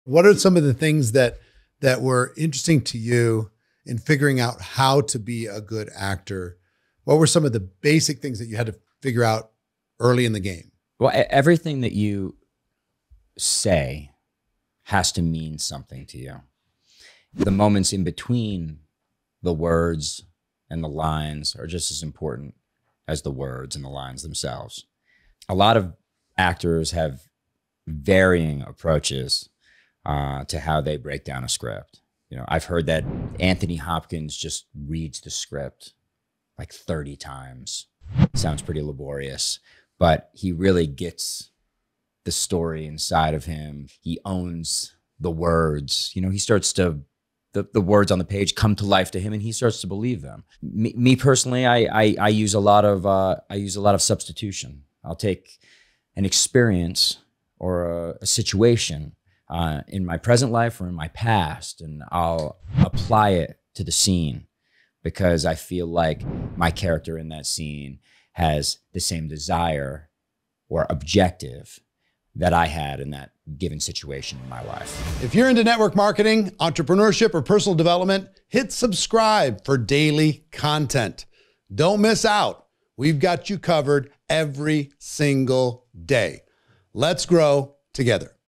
How to Be a Good Actor
Ever wonder what goes into acting? Jesse Metcalfe shares some of the methods he uses in the acting process.